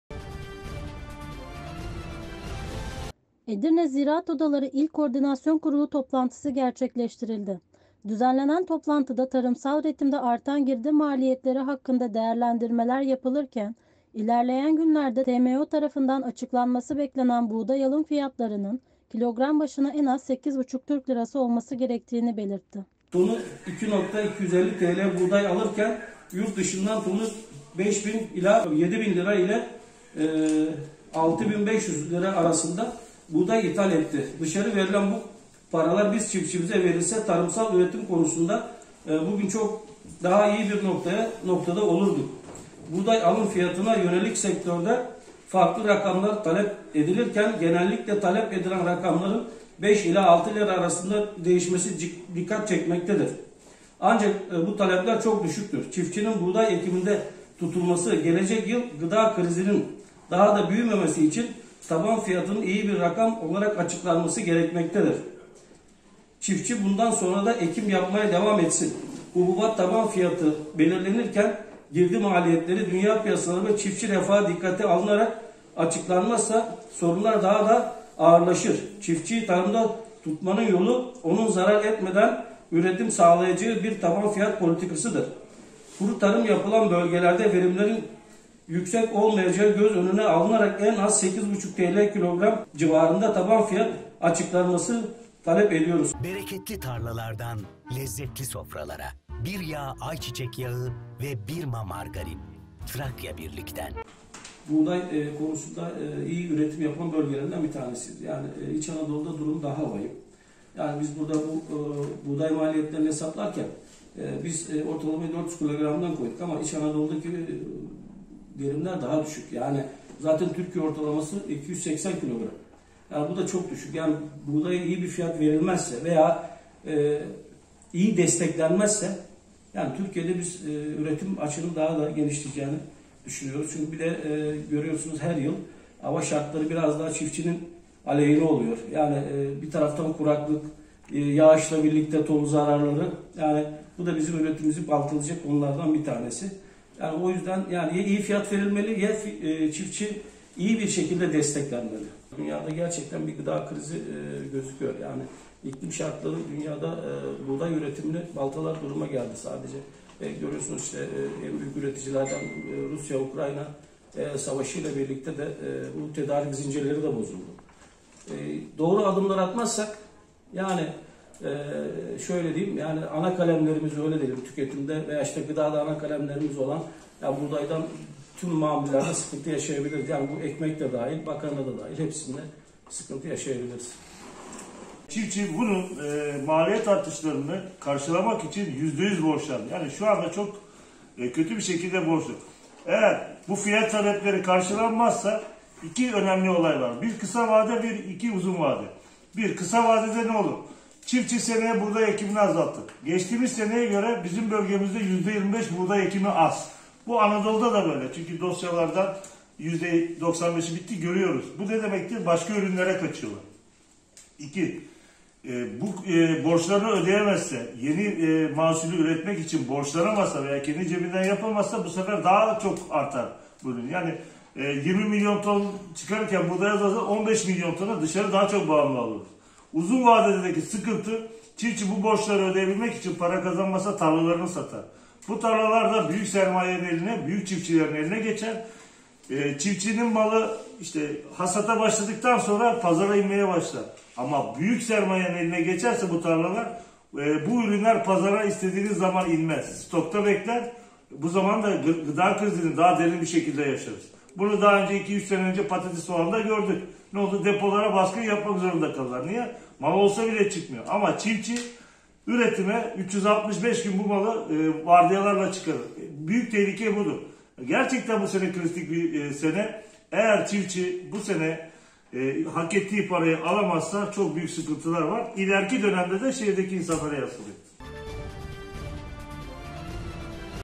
Üreticinin Beklentisi 8,5 Türk Lirası
Edirne Ziraat Odaları İl Koordinasyon Kurulu toplantısı gerçekleştirildi. Düzenlenen toplantıda tarımsal üretimde artan girdi maliyetleri hakkında değerlendirilmeler yapılırken ilerleyen günlerde TMO tarafından açıklanması beklenen Buğday alım fiyatının kilogram başına en az 8,5 Türk lirası olması gerektiği belirtildi.